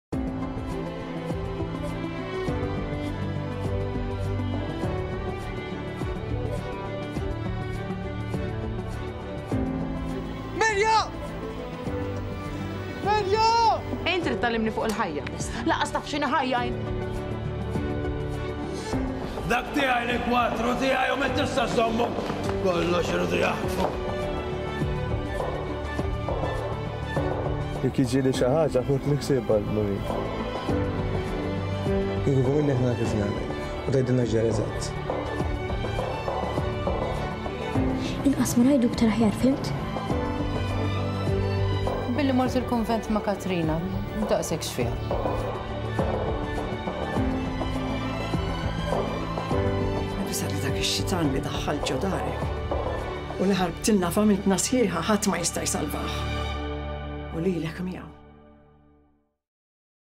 Watch the greatest Maltese productions on TOKIS with GO TV
We know how much you love to watch local productions. The greatest Maltese movies and series are at your fingertips, all in one place with TOKIS TV add-on. Explore today